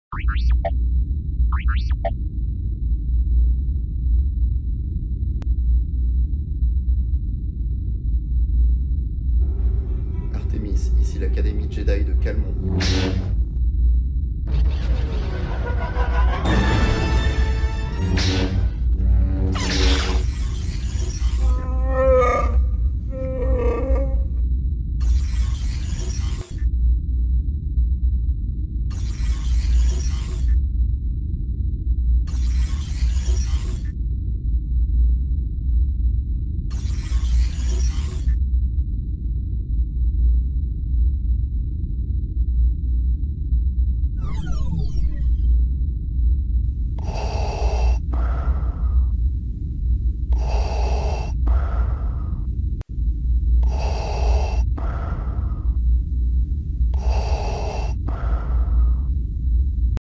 Escape game Le sauvetage
Guerre des étoiles à Calmont. Jeu de Thomas Héniart.